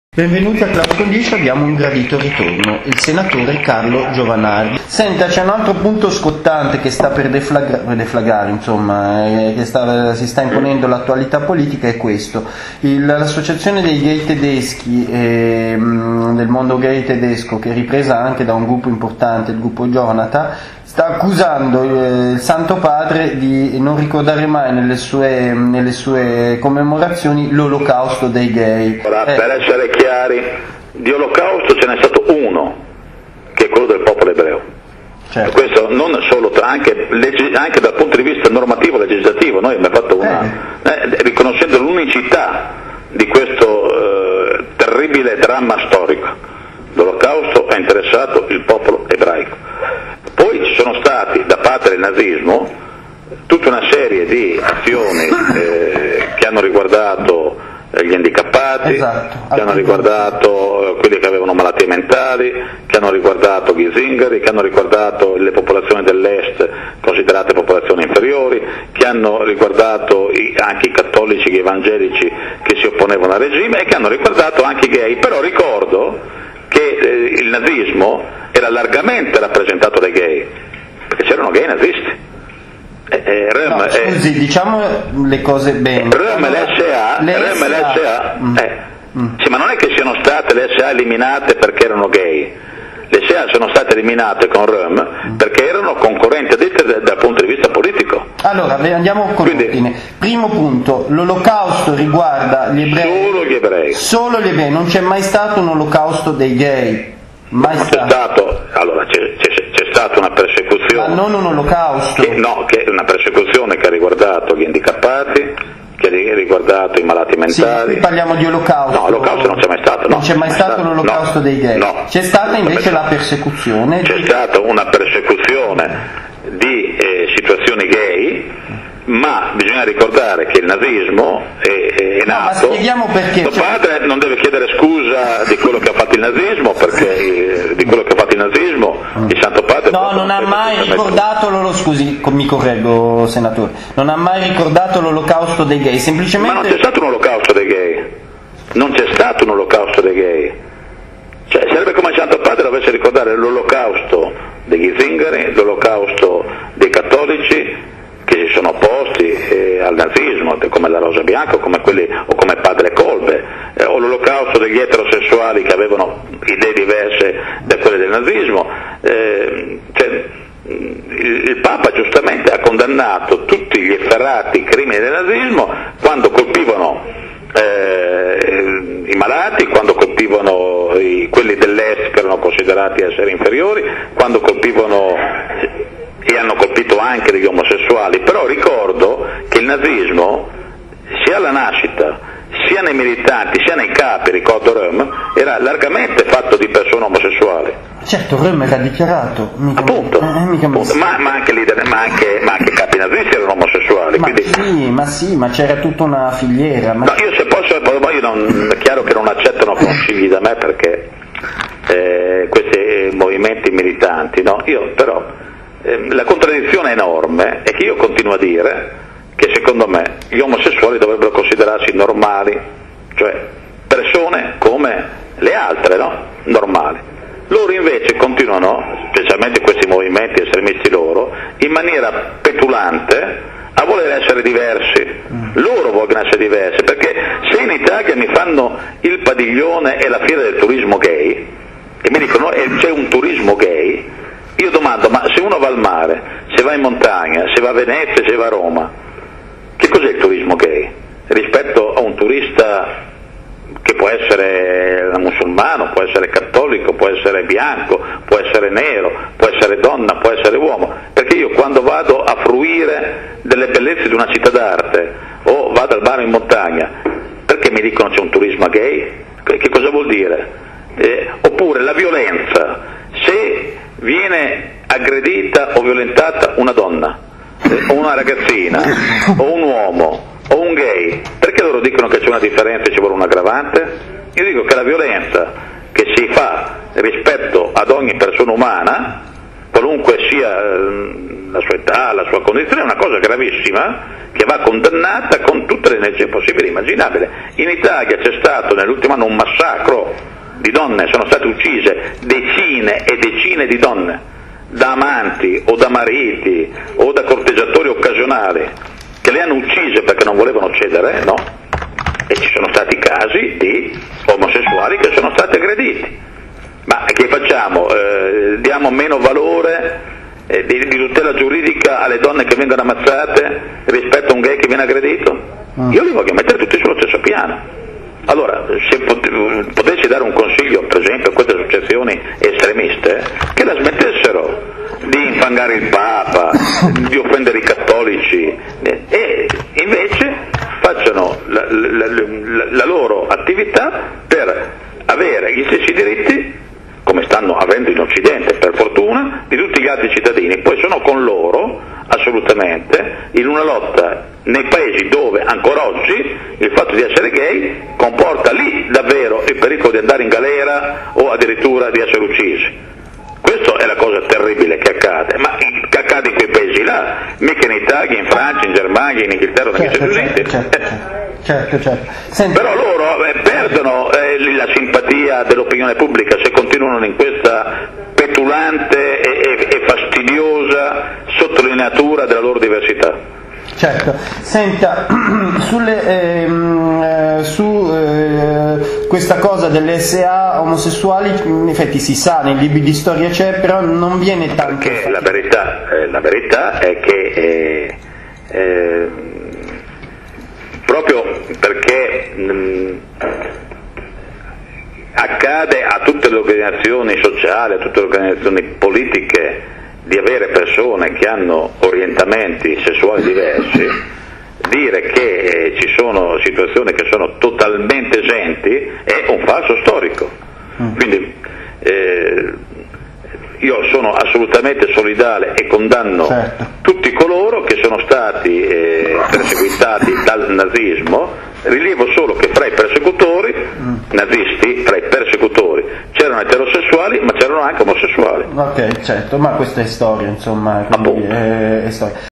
GIOVANARDI (PDL): L 'OLOCAUSTO DEI GAY NON C'È MAI STATO
Klaus Davi intervista l'Onorevole Carlo Giovanardi